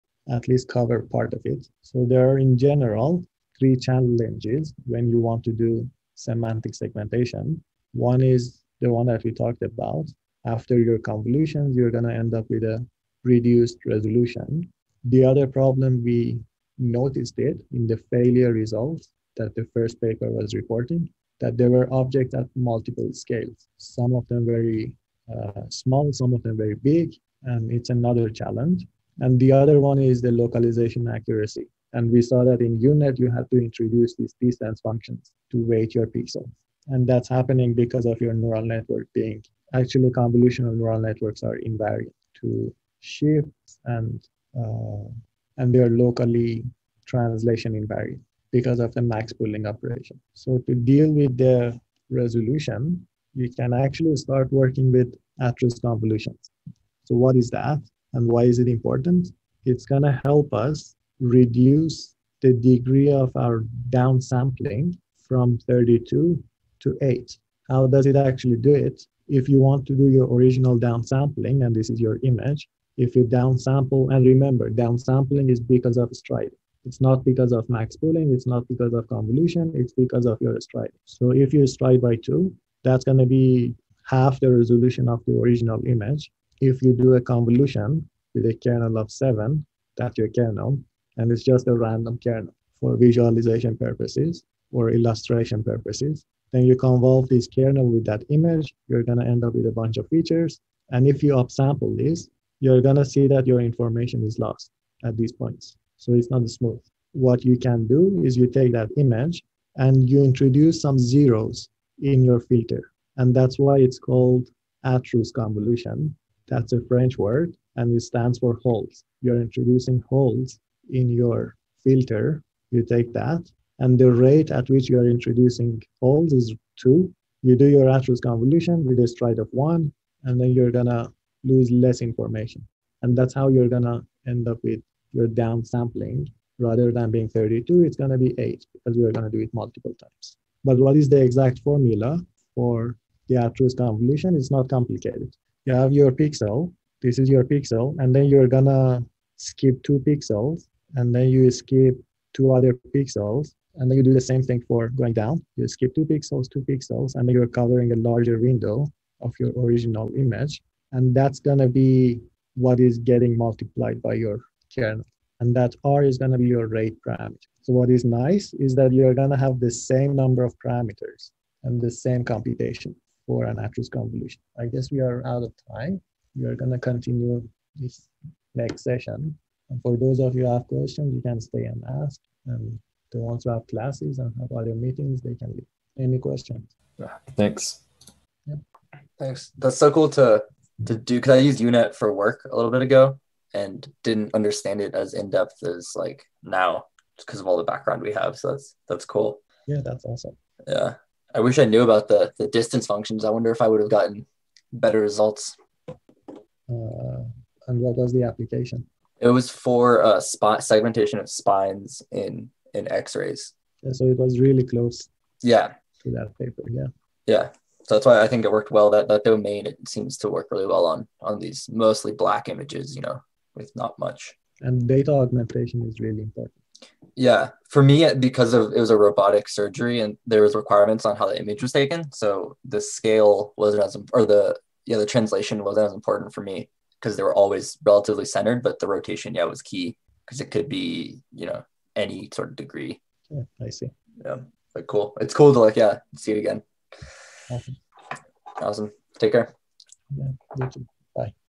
DeepLab | Lecture 30 (Part 3) | Applied Deep Learning
DeepLab: Semantic Image Segmentation with Deep Convolutional Nets, Atrous Convolution, and Fully Connected CRFs